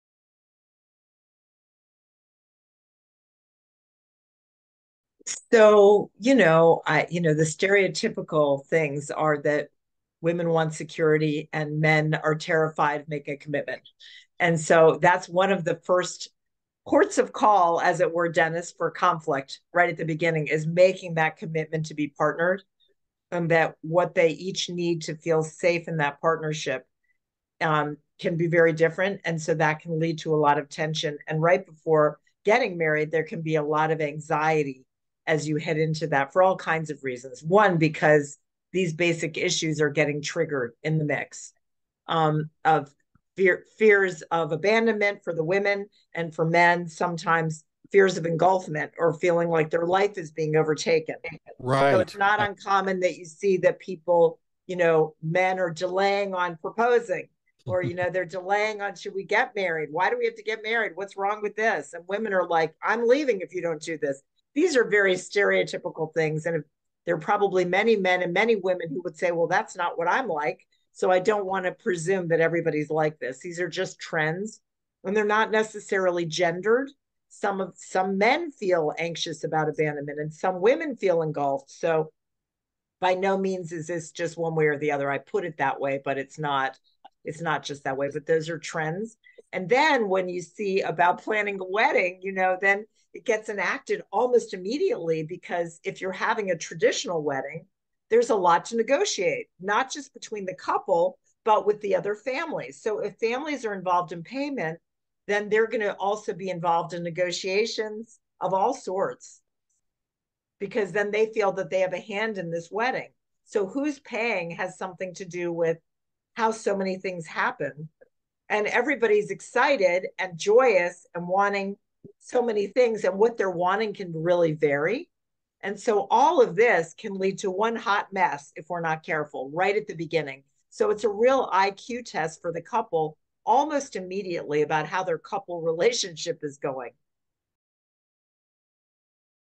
Common stressors involved with wedding planning
Dr. Cynthia Post discusses common stressors involved with wedding planning for men and women.